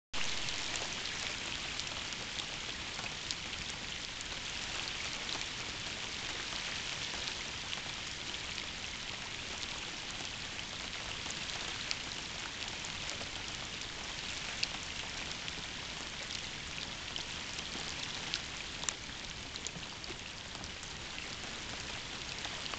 Rainy day paddle
Out for a spring kayak on Lower Scott's Flat and caught in a beautiful little rainstorm.